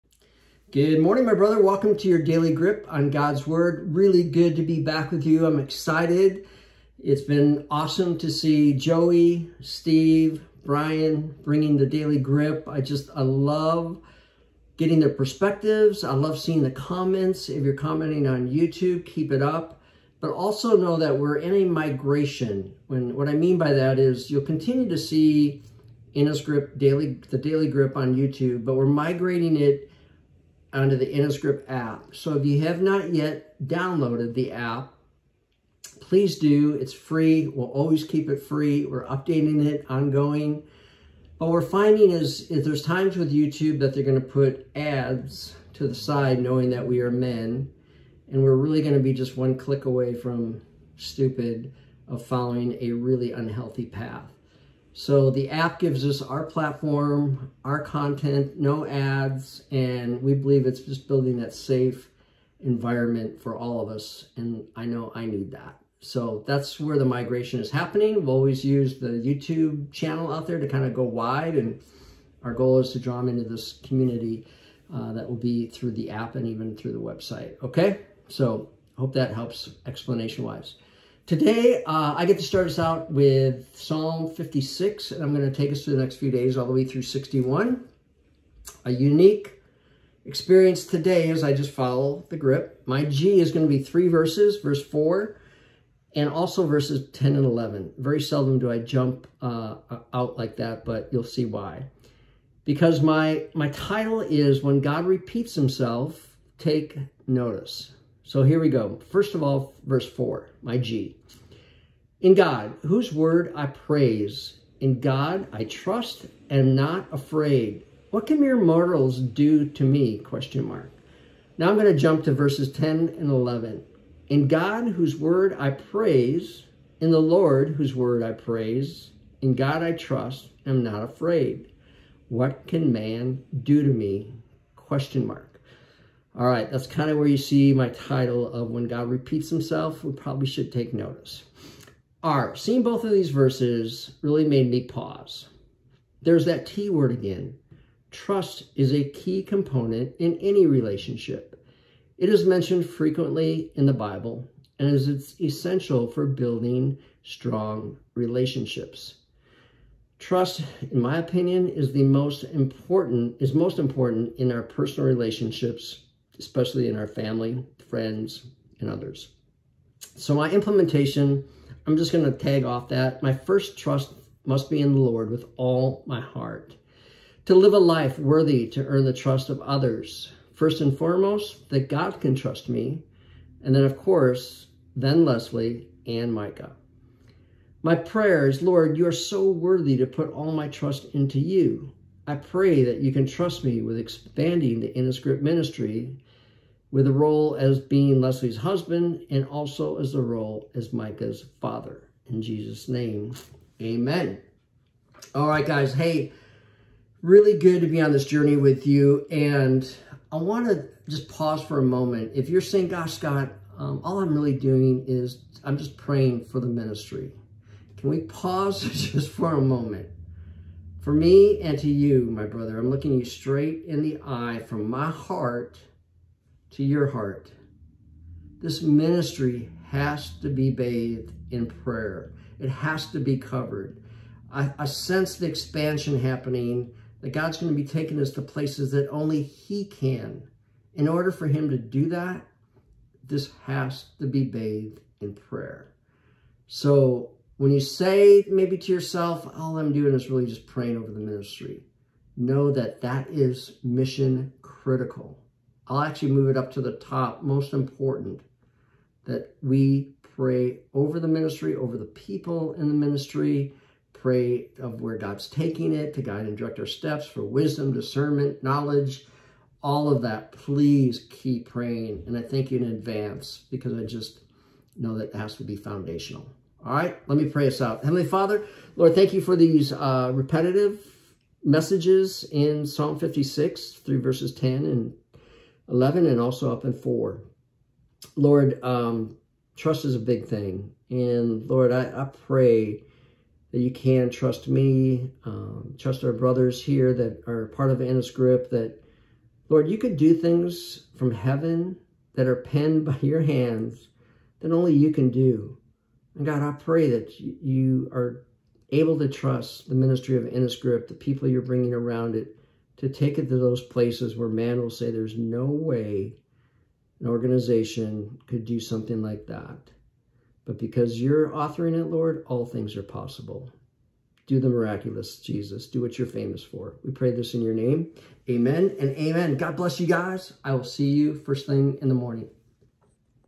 Psalm 56: When God Repeats Himself Take Notice
Join us today as Scott Lehman walks through our daily GRIP on Psalm 56.